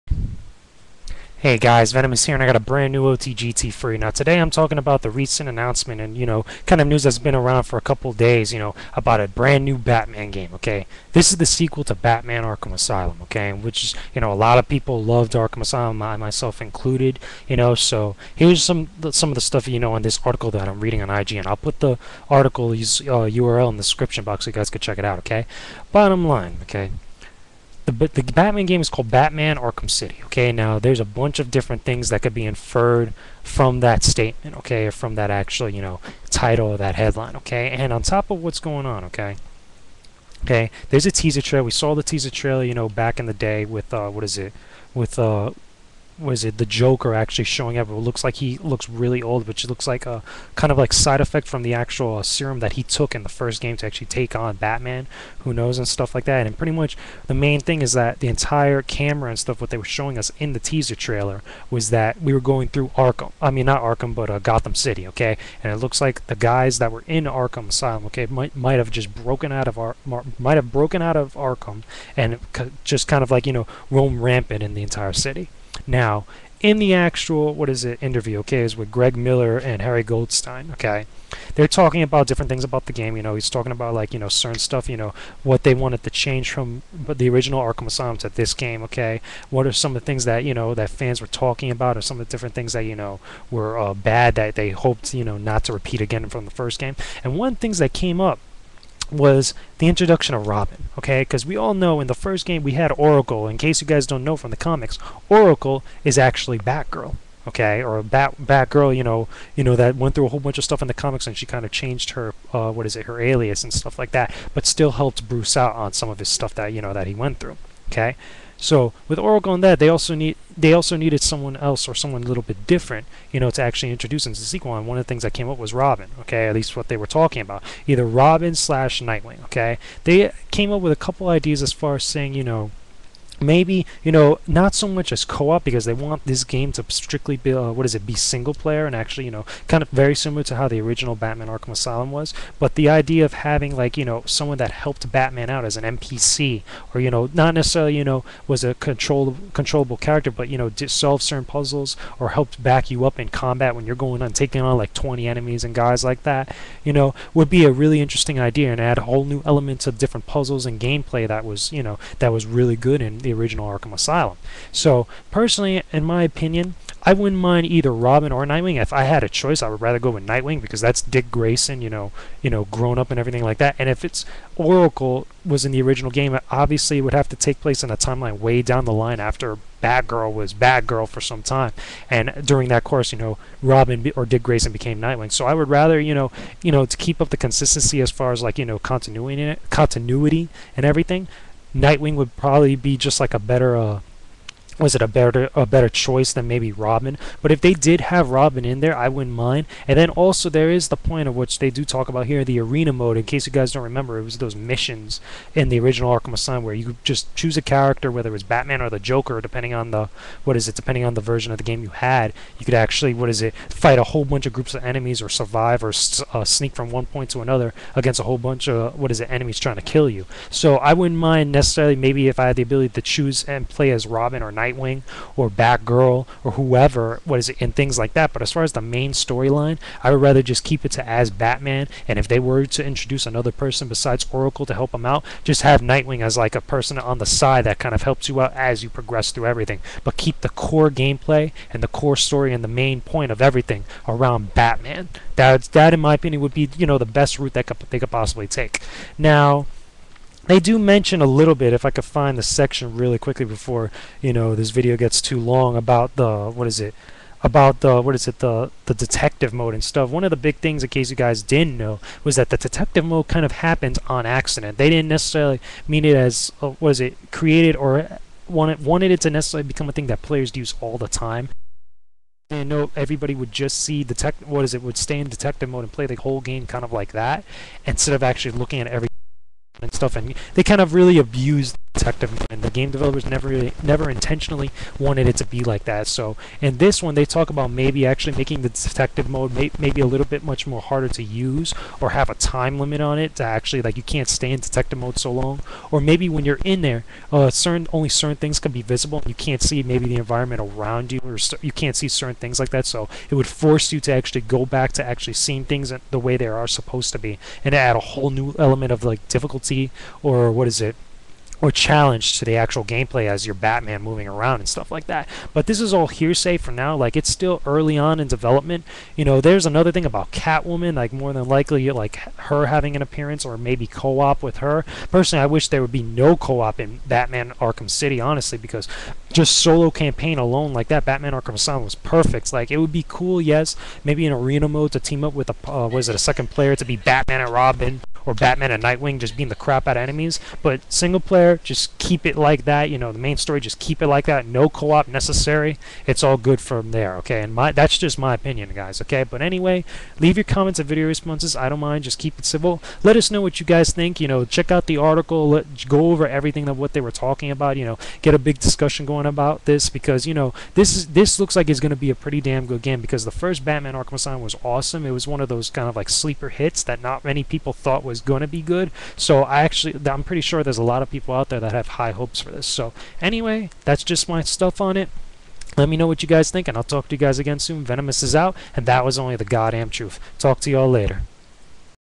OTGT---- Batman Arkham City Announcement/Interview
My thoughts on the news about Batman's next big game.